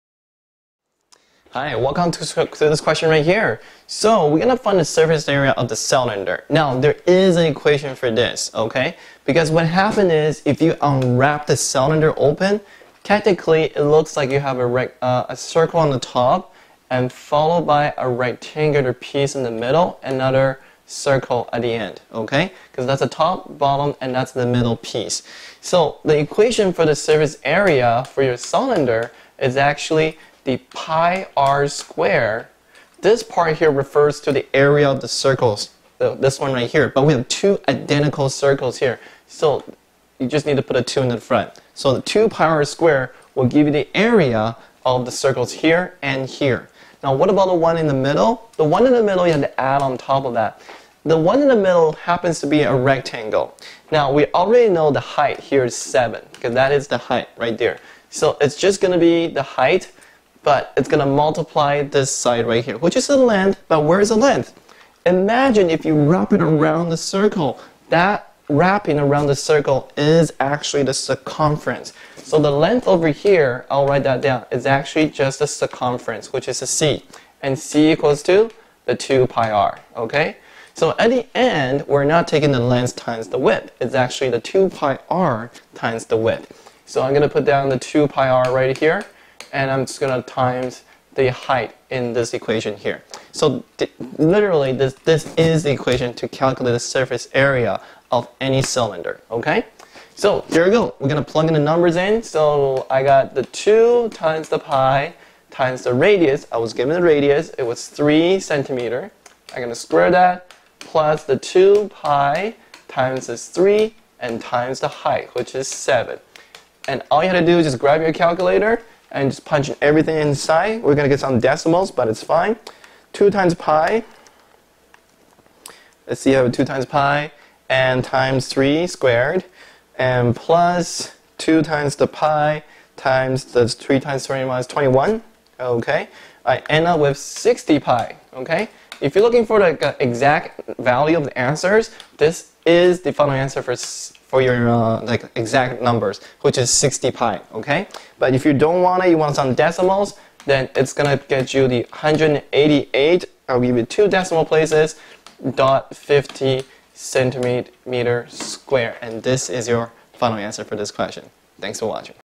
ʕ•ᴥ•ʔ Surface Area of a Cylinder - Understand in 3 minutes
Quickly master how to find the surface area of a cylinder.

As long as we have the diameter or radius and the height of the cylinder, we are able to find its surface area in no time. The surface area of a cylinder is basically adding the surface are of a rectangle and two circles together. The most difficult part is to find the circumference of the circles, which is also one of the sides of the rectangle.

Hi. Welcome to this Question Here.

how do you find the volume of a cylinder?
There is a formula for volume of a cylinder.
I will show you a volume of a cylinder formula and the sfrface area of a volume cylinder.
I will also show you how to find the surface area of a cylinder.